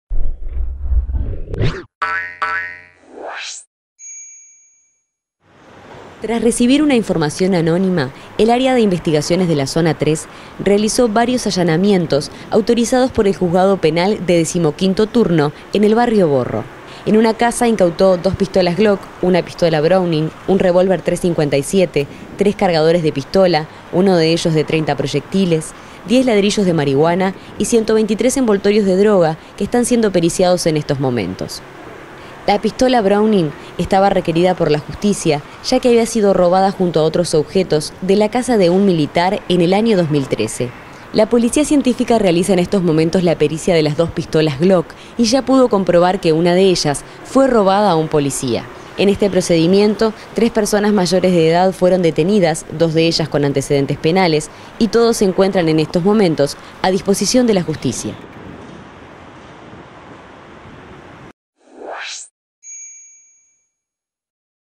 Incautan drogas y armas en barrio Borro
Tras recibir una información anónima, el área de investigaciones de la zona III realizó varios allanamientos (autorizados por el Juzgado penal de 15º turno) en el barrio Borro. En una casa incautó dos pistolas Glok, una pistola Browning, un revólver 357, 3 cargadores de pistola, uno de ellos de 30 proyectiles, 10 ladrillos de marihuana y 123 envoltorios de droga (la misma se está periciando en estos momentos).